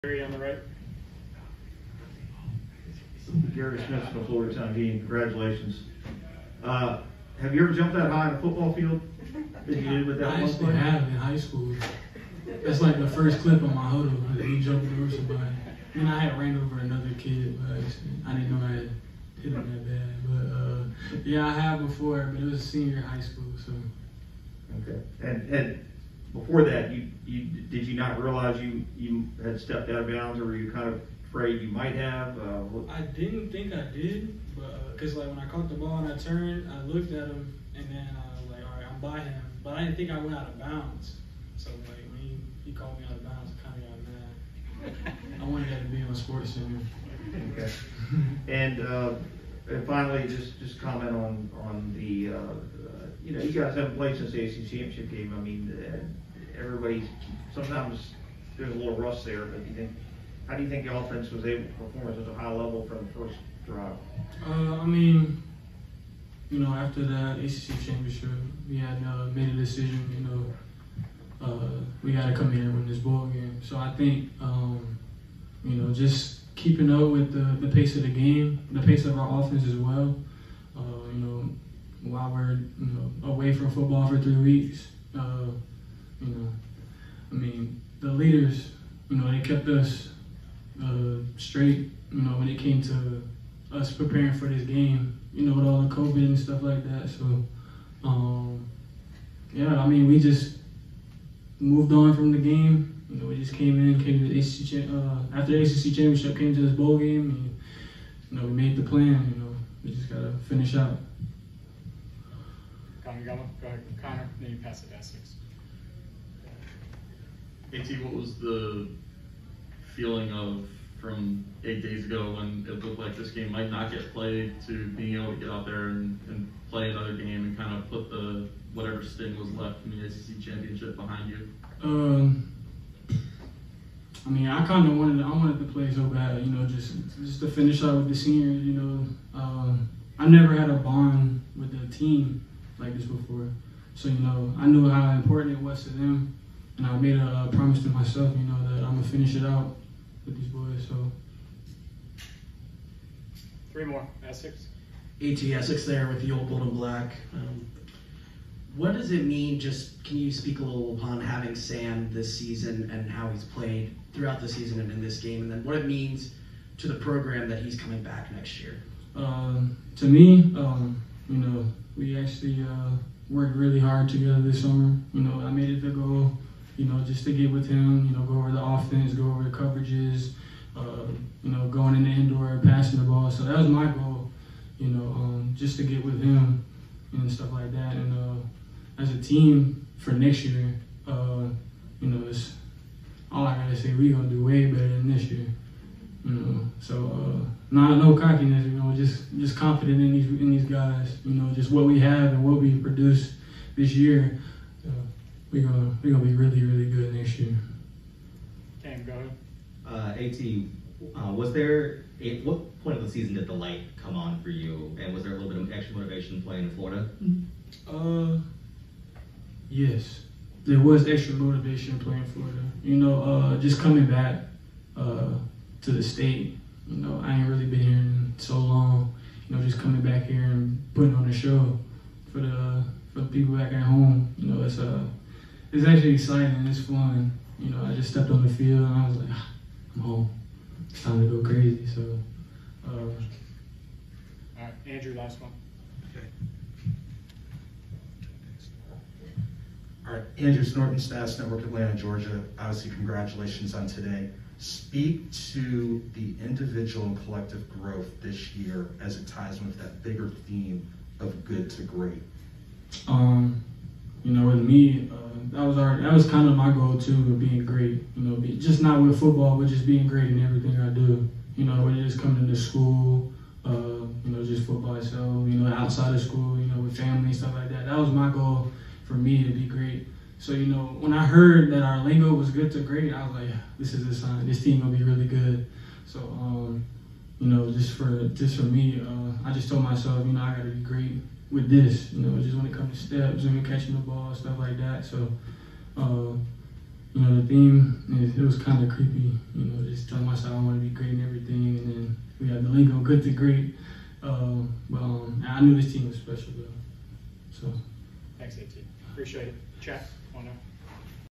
Post game with AT Perry of Wake Forest (Gator Bowl 12/31/21)
Wideout AT Perry provides reflections on the game and season after Wake’s 38-10 win over Rutgers in the Gator Bowl.  His growth is a key reason for the team’s 11-3 season.